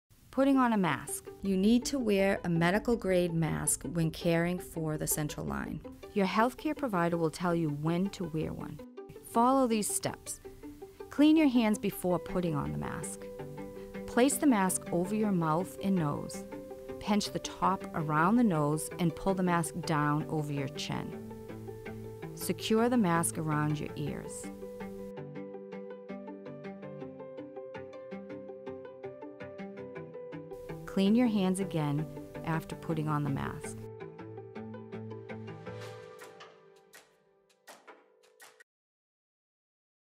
Central Venous Catheter Care: How to Put on a Mask | Boston Children’s Hospital
You will need to wear a medical mask while working with the central venous line (CVL) to keep germs from spreading from your mouth. This video is part 3 of our Central Venous Catheter Care video series and outlines the proper way to put on a mask before caring for the central venous line (CVL).

To learn more about central venous line (CVL) insertion please